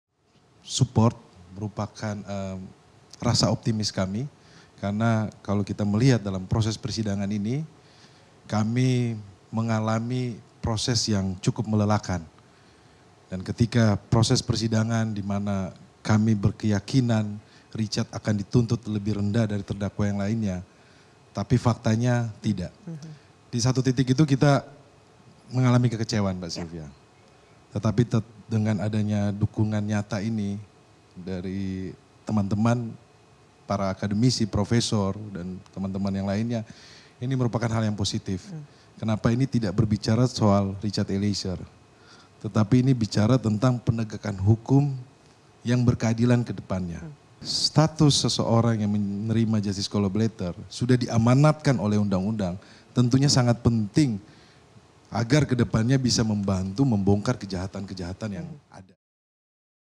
Support Jadi Rasa Optimis Ronny Untuk Tuntutan Eliezer
MetroTV, Dukungan dari masyarakat, pakar hukum dan pihak-pihak lain sangat membuat kami optimis terhadap tuntutan yang akan diberikan oleh eliezer sebagai JC